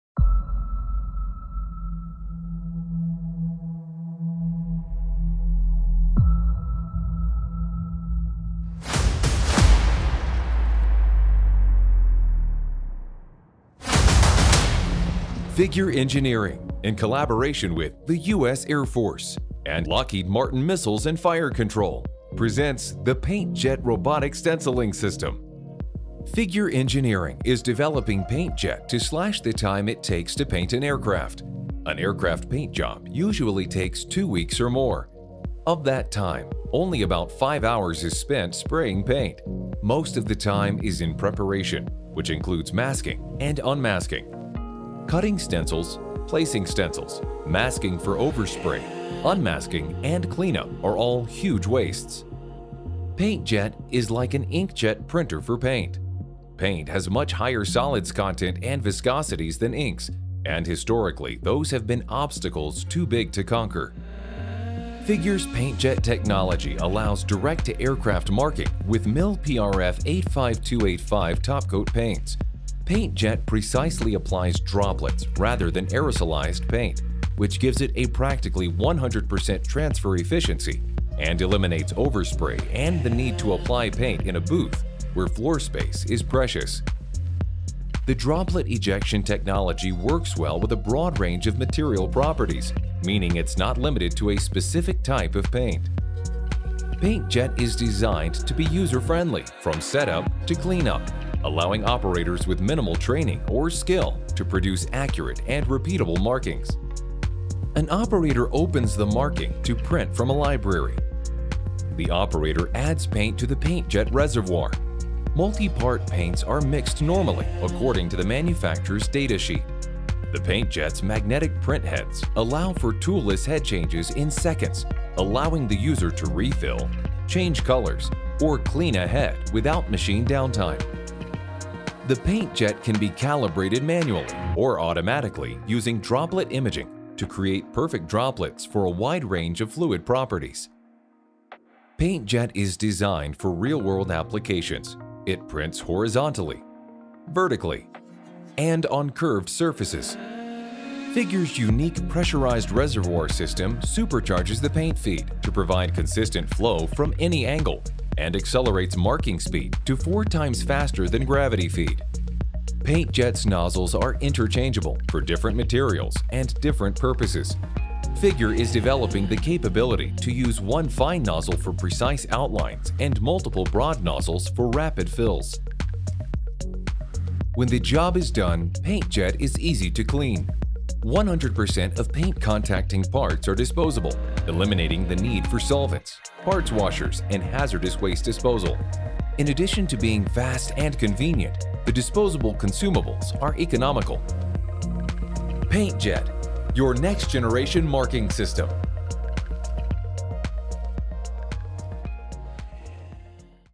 PaintJet Robotic Paint Stenciling for MIL-PRF 85285 Topcoats by Figure Engineering (Alpha-Prototype)
In collaboration with the US Air Force, Figure Engineering has developed this alpha-prototype PaintJet robot to automate the time- and labor-intensive marking of aircraft and aerospace assets.  This PaintJet alphas prototype demonstration is printing horizontally, vertically, and on complex curved surfaces with MIL-PRF 85285 topcoat at the standard mix ratio of 3:1:1.